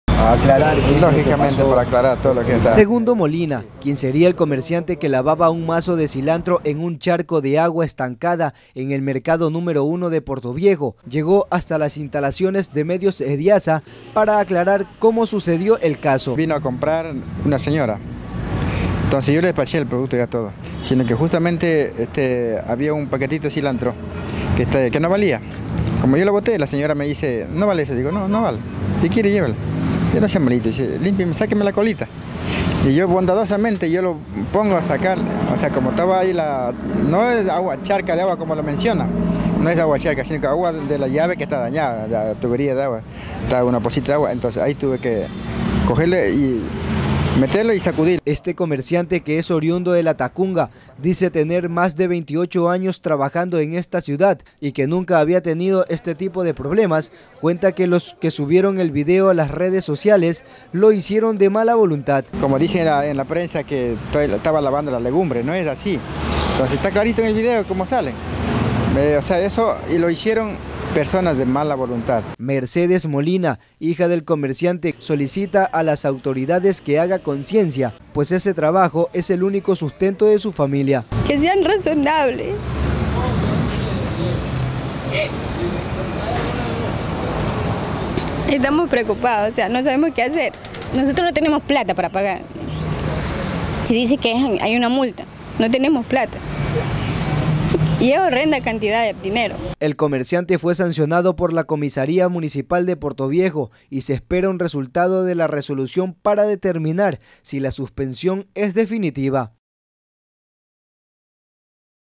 Portoviejo: Comerciante sancionado aclara porqué lavó el cilantro en charco
Portoviejo